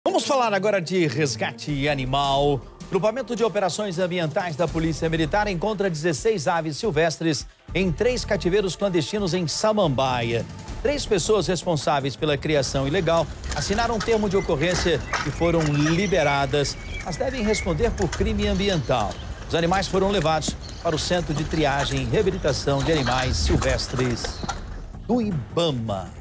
Polícia resgata 16 pássaros silvestres em Samambaia
O BPMA (Batalhão de Polícia Militar Ambiental) resgatou 16 aves silvestres em três cativeiros clandestinos em Samambaia. Três pessoas responsáveis pela criação das aves assinaram o termo de ocorrência e foram liberadas. Eles devem responder por crime ambiental. Os animais foram levados para o Cetas (Centro de Triagem e Reabilitação de Animais Silvestres) do Ibama.